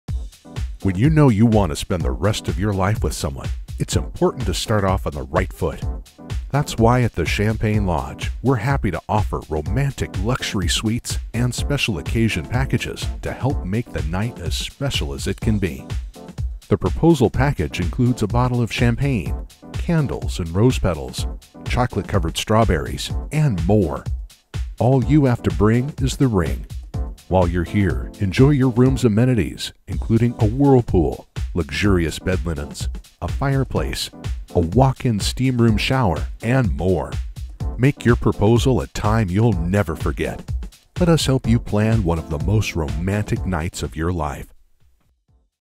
Romantic Hotel Suite with Marriage Proposal Package
When you’re ready to pop the question, let the Champagne Lodge help—you’ll love our marriage proposal package!

Proposing to the one you love should be a night to remember, and when you book a stay at the Champagne Lodge, you can be sure you’ll never forget it. Our marriage proposal package includes champagne and champagne flutes, candles and rose petals, balloons, chocolate covered strawberries and a gift certificate to a nearby upscale restaurant, and much more. Make the most romantic night of your life even better by spending it at the Champagne Lodge.